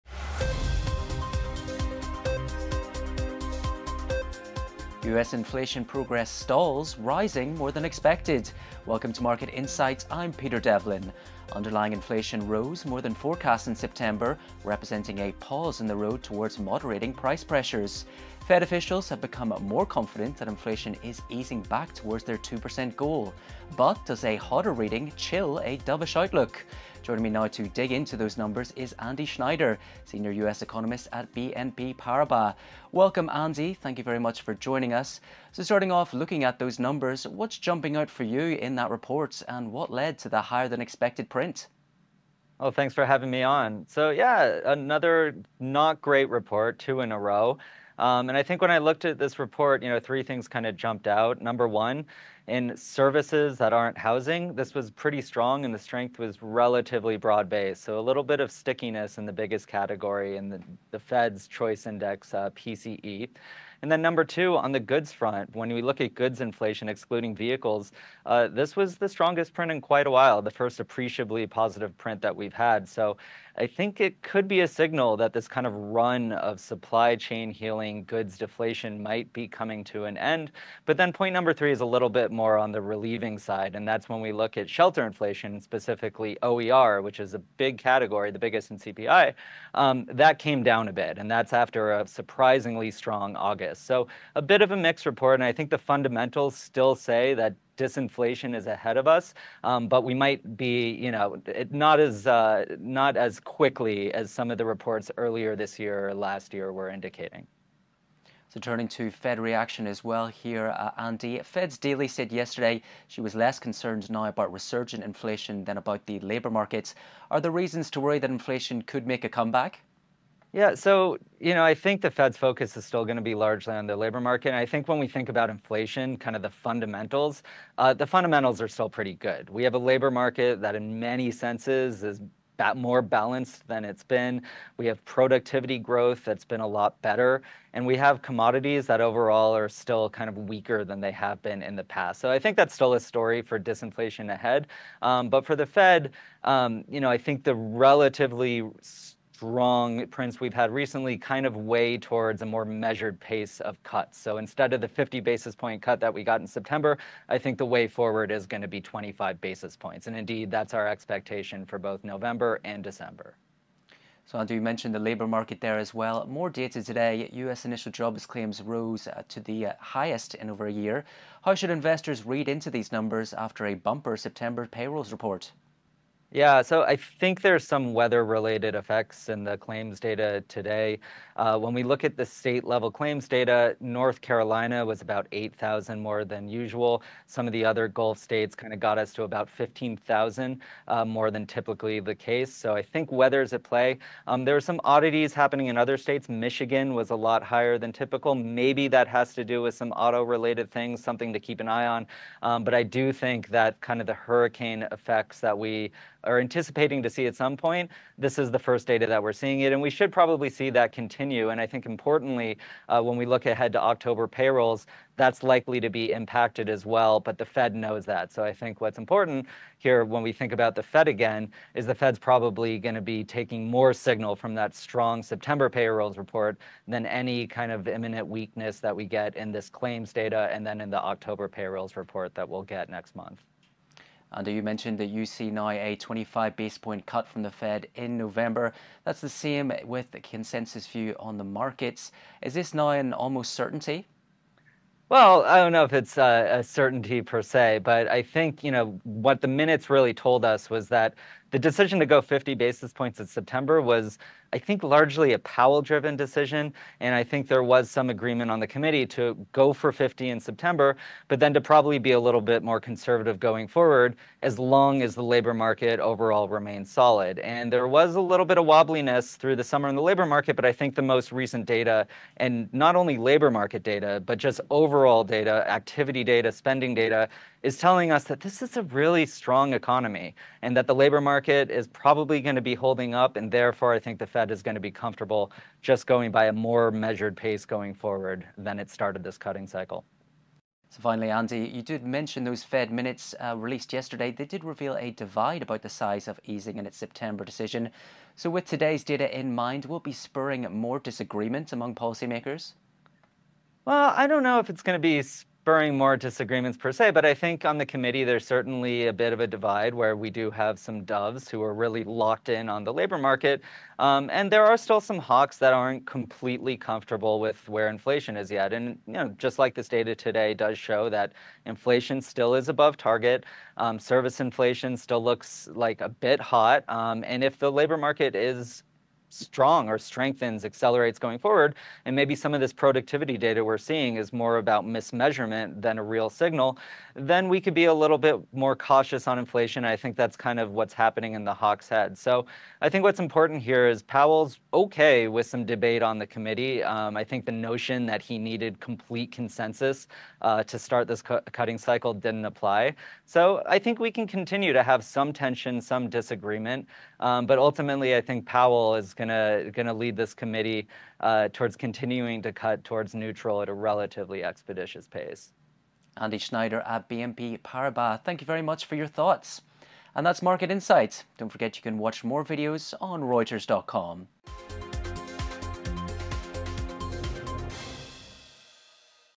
Market Insight: Divide among Fed policymakers to persist | REUTERS
US inflation was hotter than expected in September, but a jump in weekly jobless claims shifted the focus to the need for further policy easing. BNP Paribas’ Andy Schneider told Reuters that the Federal Reserve will be comfortable with a measured pace of interest rate cuts ahead, despite debate among officials.